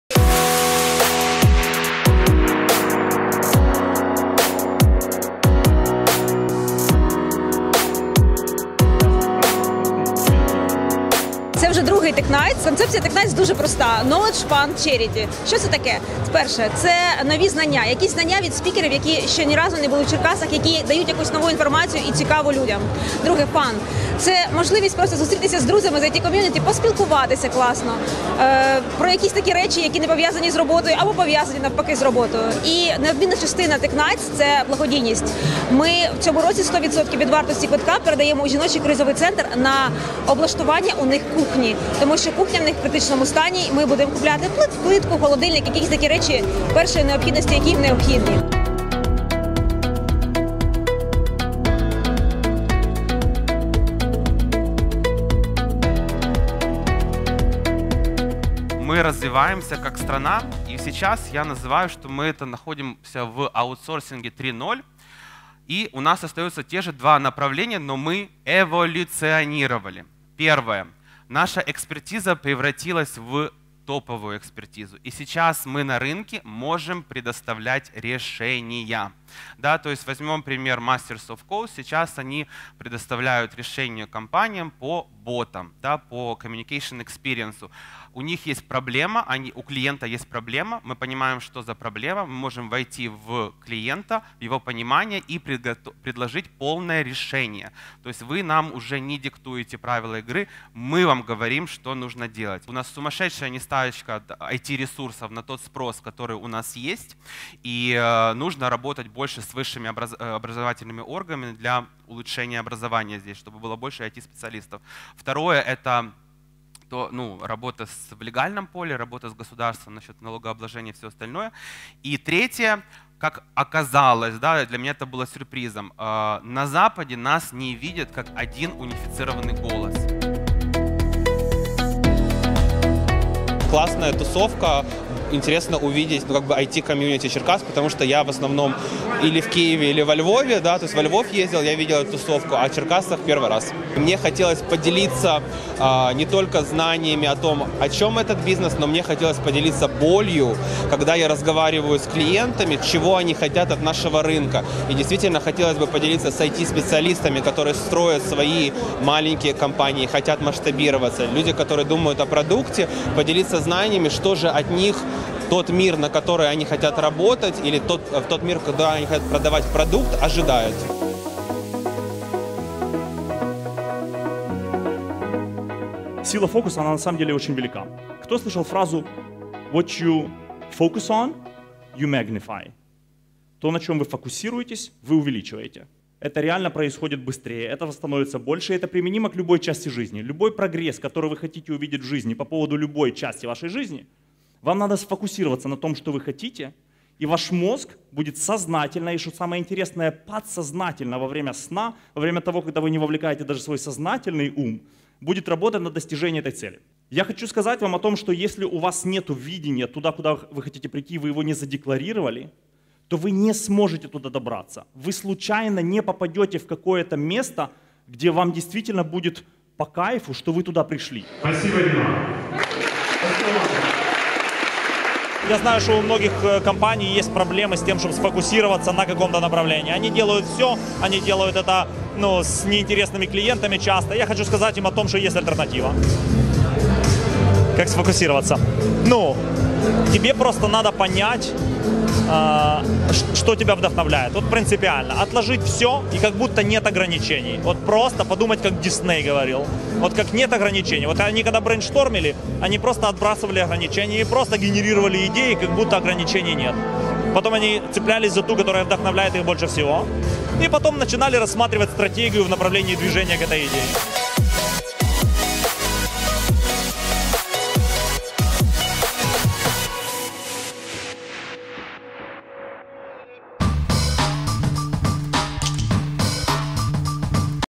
Tech Nights: черкаські айтішники збирали гроші на підтримку жіночого кризового центру
На блогодійній вечірці Tech Nights команда Master of Code зібрала близько 20 тисяч гривень для жінок, які постраждали від насильства. За ці гроші придбають кухонну техніку для кризового центру. На вечірці обговорювали нові тренди аутсорсингу та як правильно концентруватися і розвивати проект.